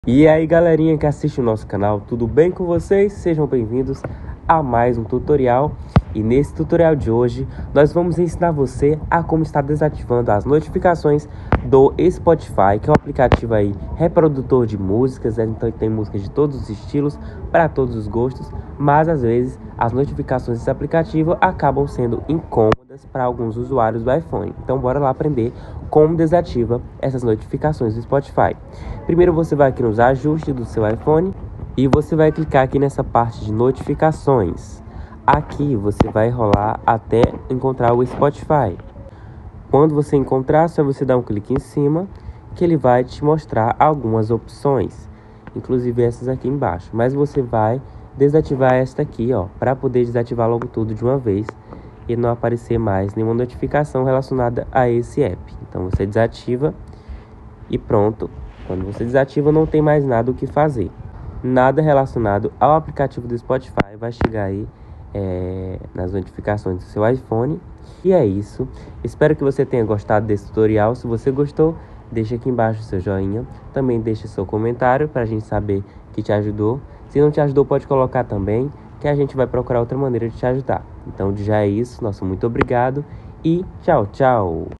Como DESATIVAR AS NOTIFICAÇÕES DO SPOTIFY no IPHONE!
Olá pessoal, beleza. Sejam todos bem - vindos a mais um tutorial do nosso canal. Neste vídeo lhes ensinaremos a desativar as notificações do Spotify no iphone!. Confira!